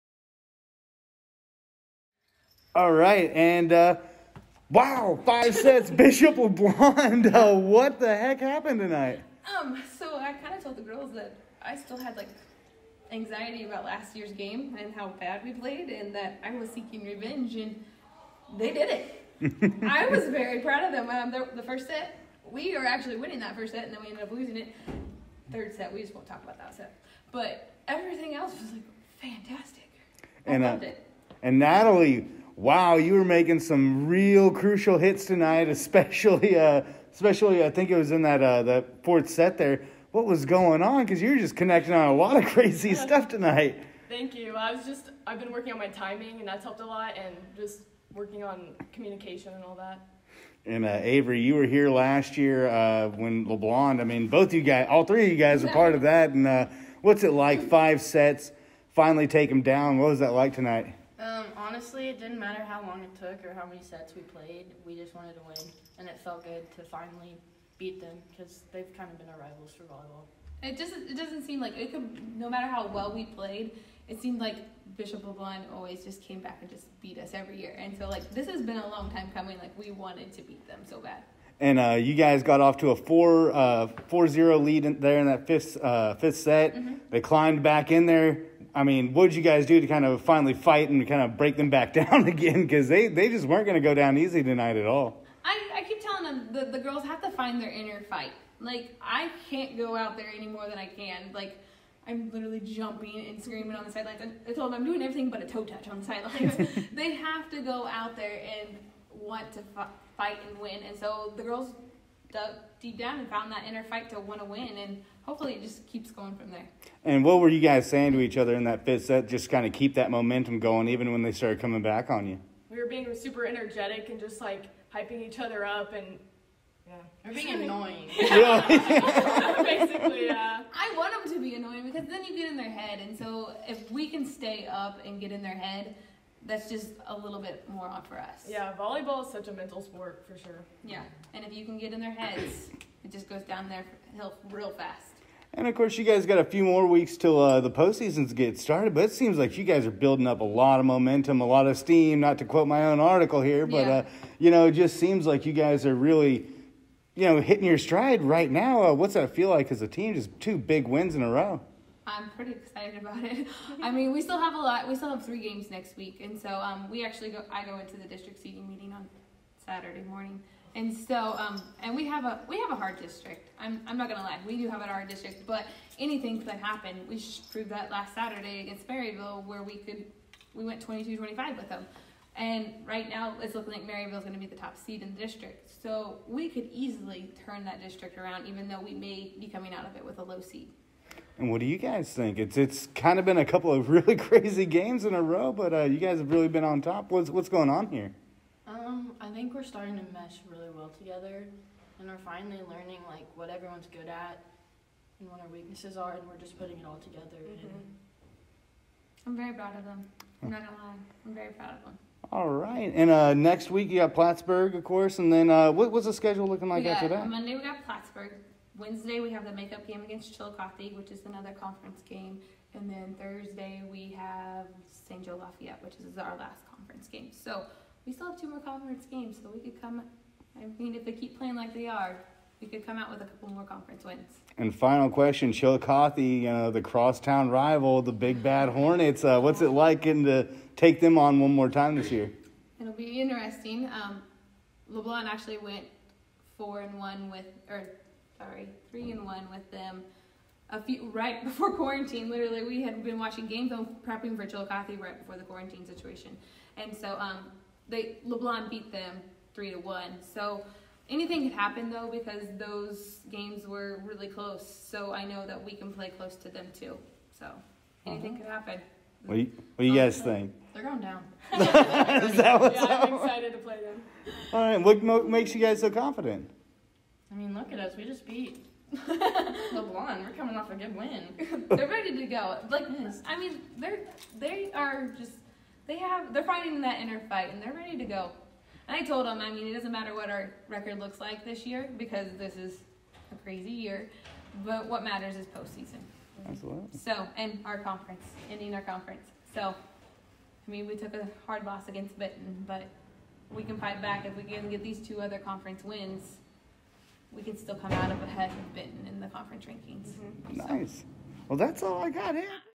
Lady Dragons upset LeBlond 2020
Cameron coach Addie Floyd, junior setter Natalie Garr and junior outside hitter Avery McVicker talk about last Thursday's upset of Bishop LeBlond.